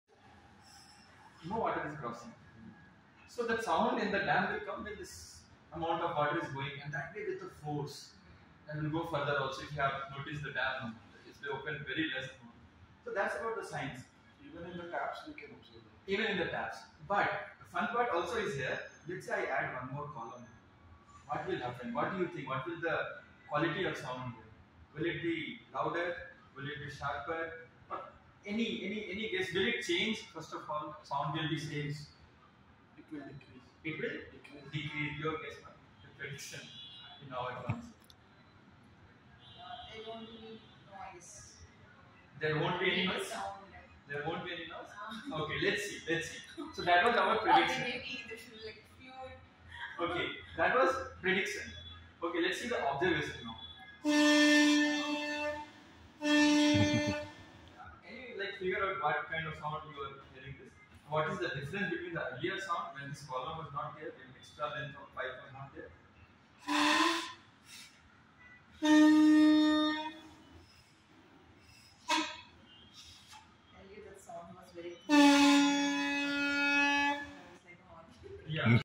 ActionAid team is enjoying the experience | ThinkTac
Experiential science activities are most effective when conducted as interactive sessions, and the ActionAid team is enjoying the experience.